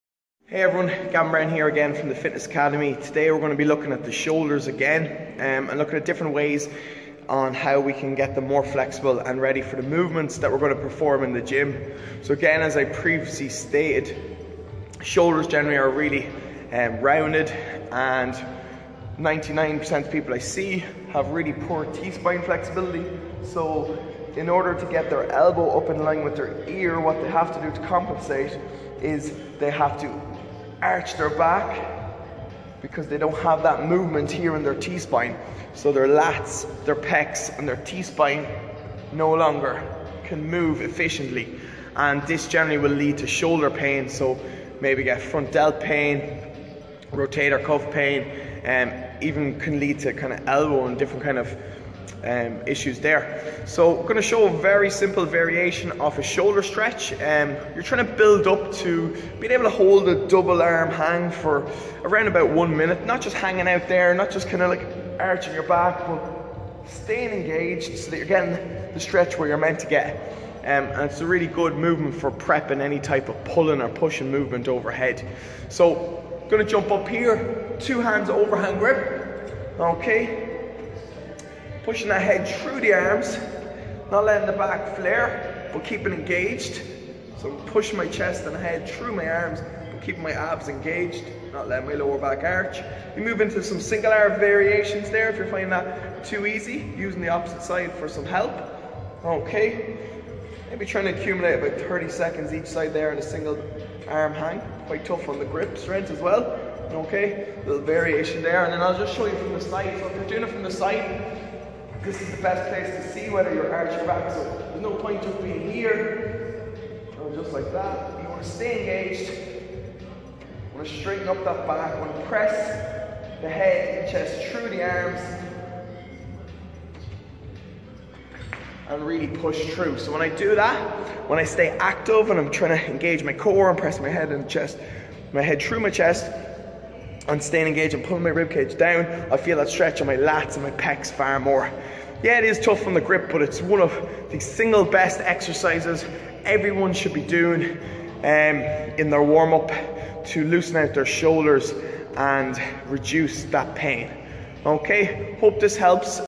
Best shoulder drill
How to gain back full function in your tight shoulders